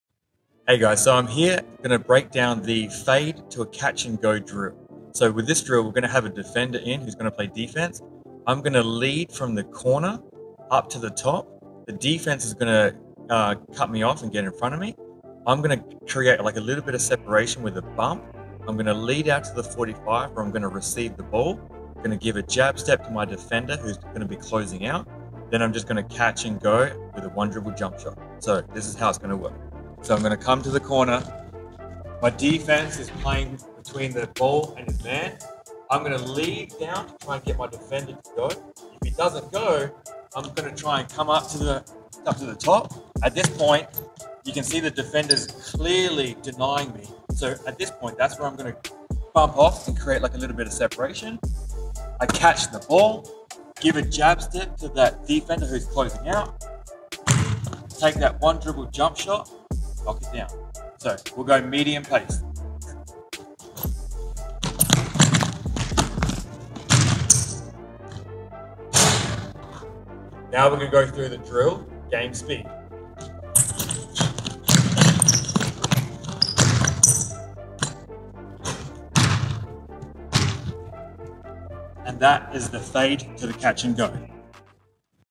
The Fade to Catch and Go Drill
In this video, we demonstrate breaking down the fade to catch and go drill.